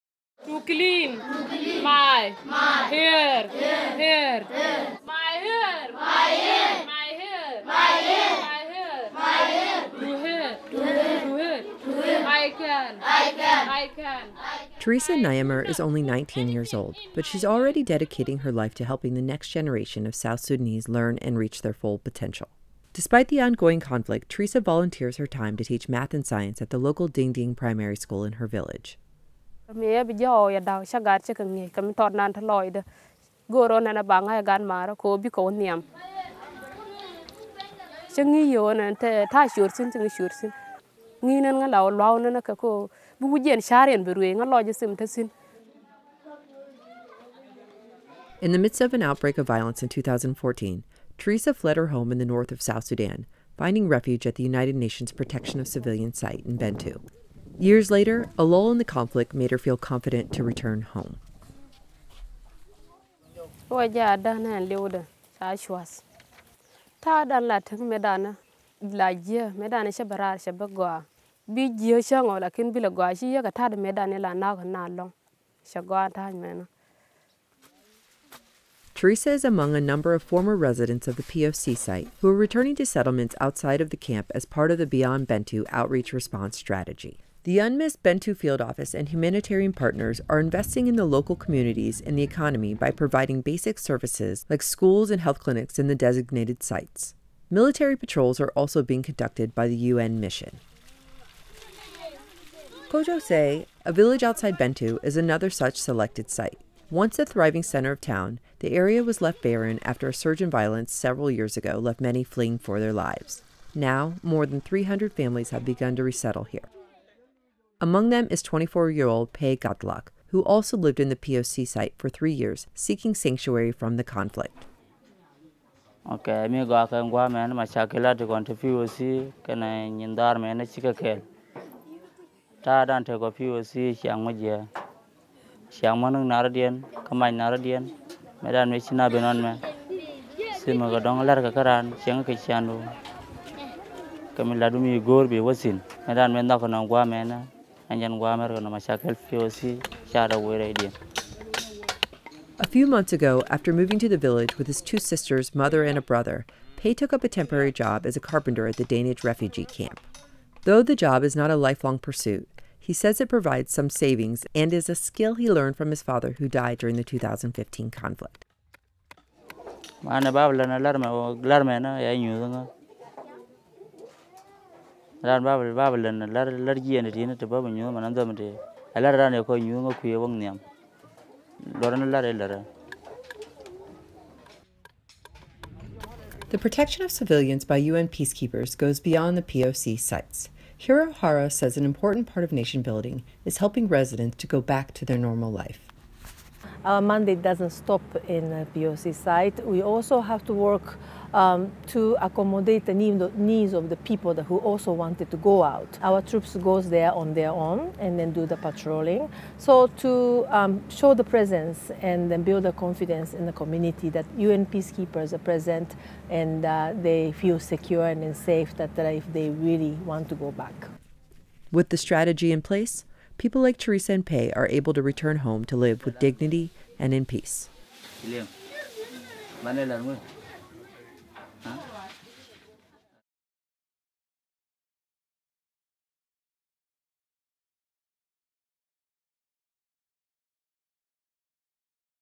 Beyond Bentiu
Local residents are leaving the safety of the Protection of Civilians Site (PoC) next to the UN base in Bentiu, South Sudan in search of a new future.  With the aid of the Beyond Bentiu strategy, the local population that wants to return to designated communities around Bentiu will have the support of humanitarian partners as well as the security of UNMISS military patrols.  Many who found refuge in the PoC site during surges in conflict are taking advantage of a lull in fighting to reestablish themselves outside the permimeter of the PoC site.  This joint approach is proving successful at Bentiu where more than 17,000 people who lived in the UN POC site have now officially returned to their homes to live with dignity, and in peace.